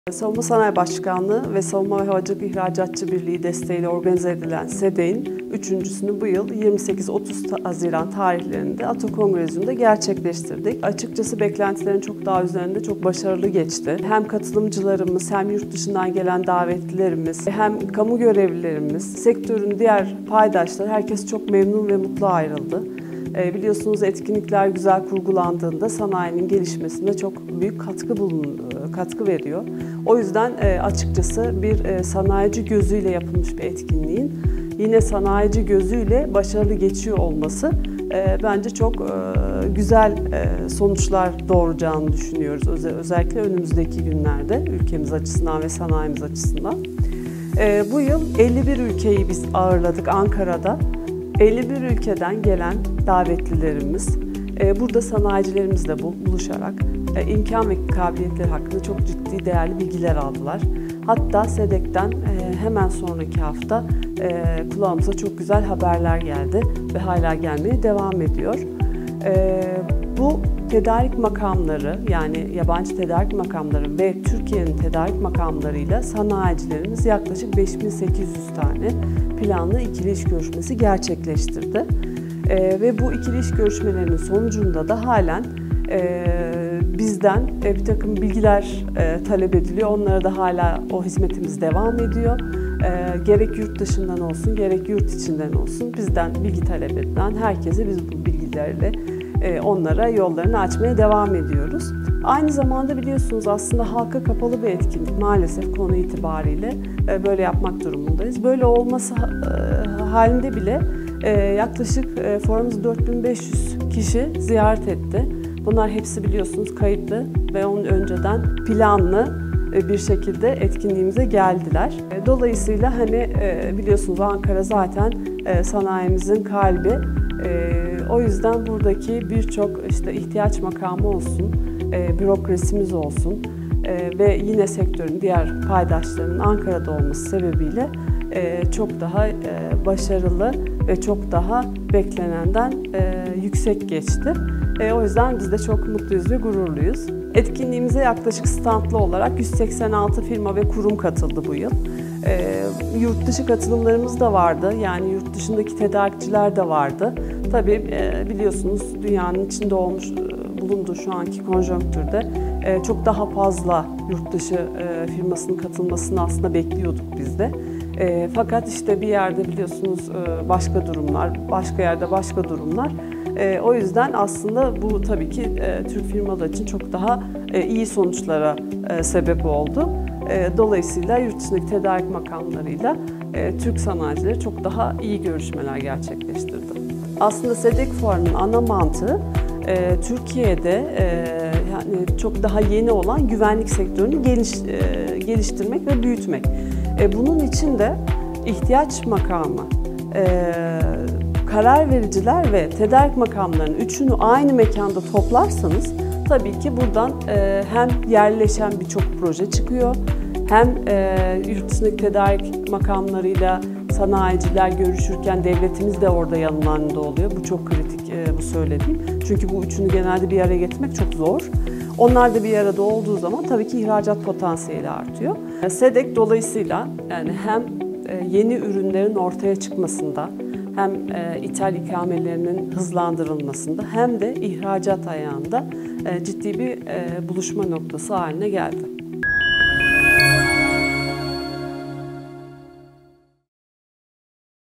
Hilal Ünal, Anayurt ve Sınır Güvenliği Fuarı SEDEC 2022'yi değerlendirdi
Anayurt ve Sınır Güvenliği Fuarı SEDEC’in üçüncüsü geçtiğimiz Haziran ayında geniş bir katılımla gerçekleştirildi.

Yurt dışında yaygın olan ancak Türkiye’de SEDEC’le birlikte duyulmaya başlanılan “Anayurt ve Sınır Güvenliği” konsepti ile hazırlanan organizasyon hakkında SEDEC Koordinatörü Hilal Ünal, Defensehere.com’a değerlendirmelerde bulundu:

“Savunma Sanayii Başkanlığı ve Savunma ve Havacılık İhracatçılar Birliği desteği ile organize edilen SEDEC’in üçüncüsünü bu yıl 28-30 Haziran tarihlerinde ATO Congresium’da gerçekleştirdik. Açıkçası beklentilerin çok daha üzerinde çok başarılı geçti. Hem katılımcılarımız hem yurt dışından gelen davetlilerimiz hem kamu görevlilerimiz, sektörün diğer paydaşları herkes çok memnun ve mutlu ayrıldı.

Biliyorsunuz etkinlikler güzel kurgulandığında sanayinin gelişmesine çok büyük katkı veriyor. O yüzden açıkçası bir sanayici gözüyle yapılmış bir etkinliğin yine sanayici gözüyle başarılı geçiyor olmasının çok güzel sonuçlar doğuracağını düşünüyoruz özellikle önümüzdeki günlerde sanayicimiz ve ülkemiz açısından.